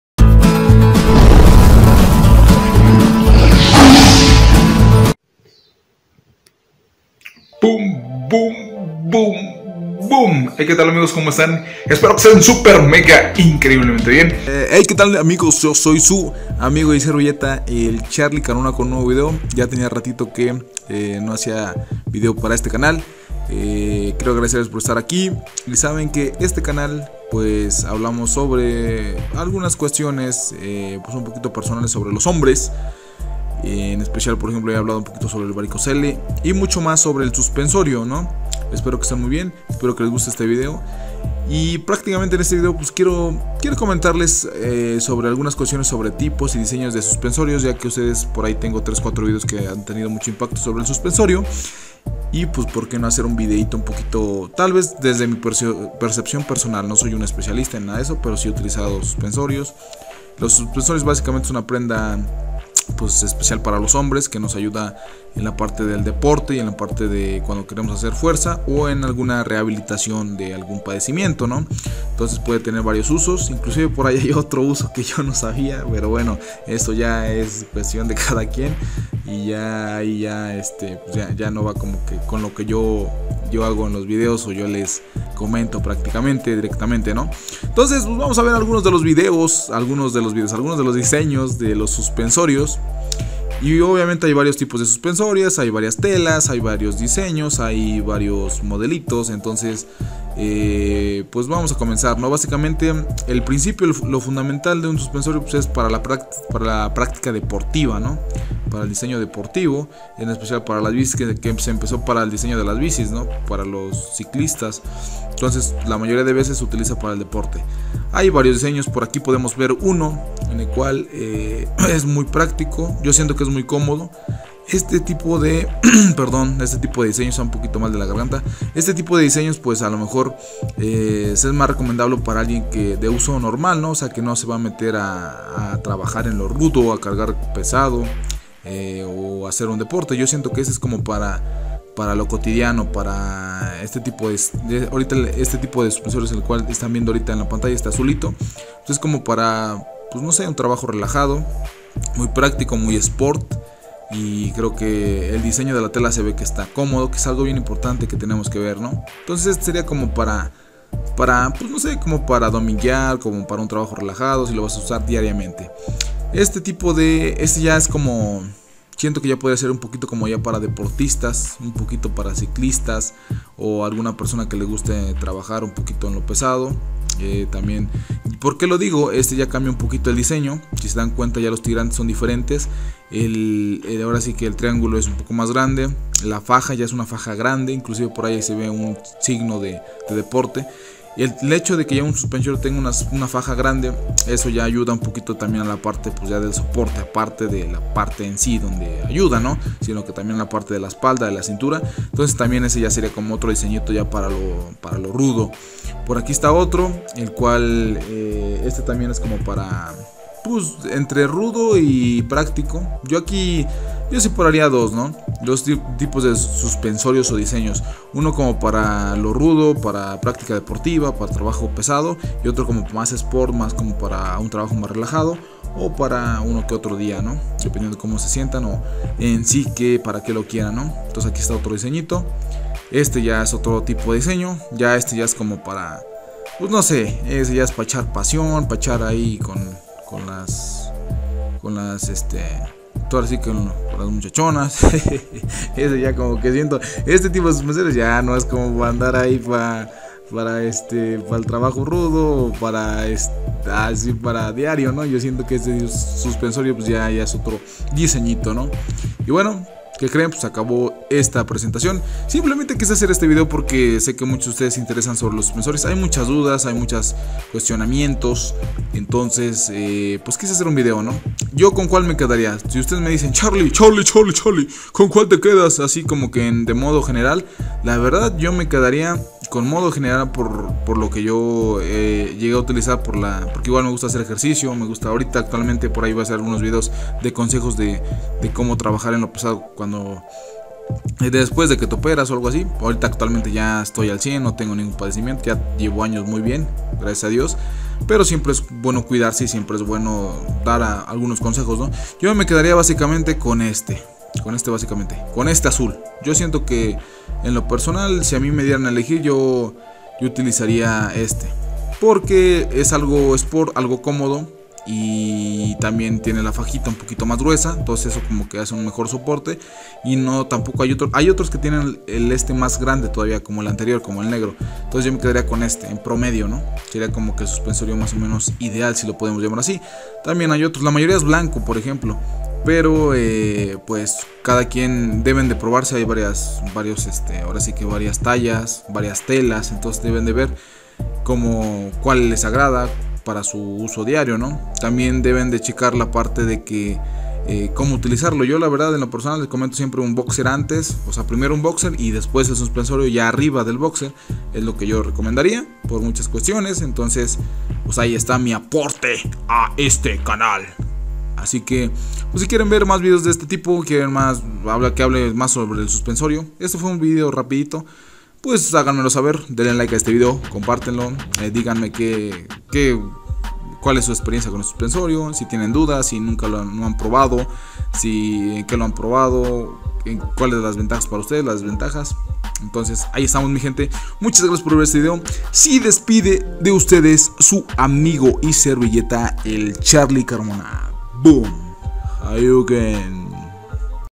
Tipos y Diseños de Suspensorios ¿Cual es mejor ? ¿Cual conviene comprar??
Tipos y Diseños de Suspensorios ¿cual es mejor el mejor suspensorio? ¿Que suspensorio conviene comprar??

Un suspensorio deportivo es, básicamente, un bolsillo de tela que se usa para mantener las esferas del dragon al cuerpo. Los suspensorios deportivos se usan más en las actividades física intensas, como el ciclismo y el levantamiento de pesas.